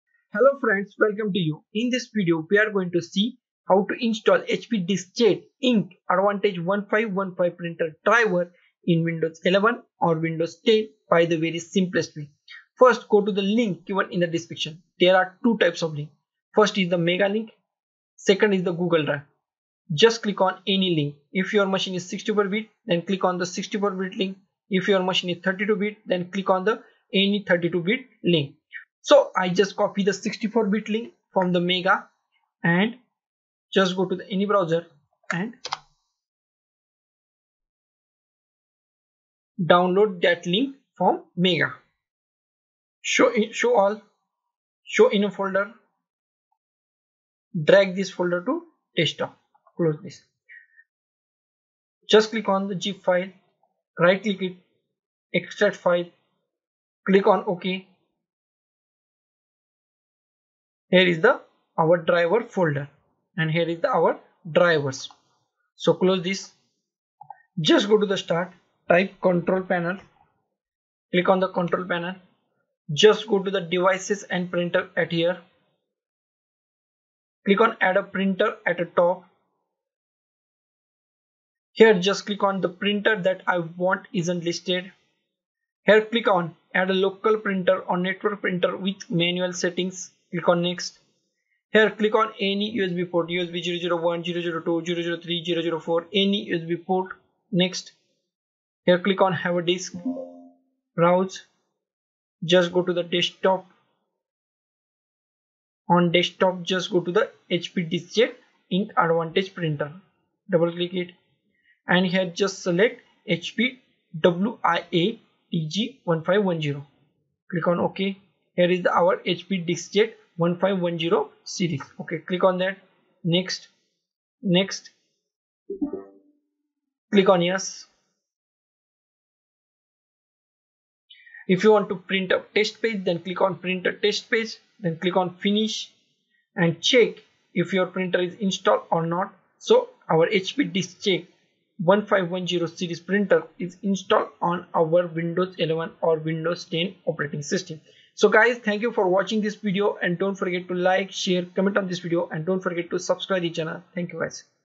How to Install HP Deskjet Ink Advantage 1515 Printer Drivers in Windows 11 - Windows 10
In this Video we are going to see How to Install HP Deskjet Ink Advantage 1515 Printer Drivers in Windows 11,  Windows 10 or any other windows Operating system by the simplest way.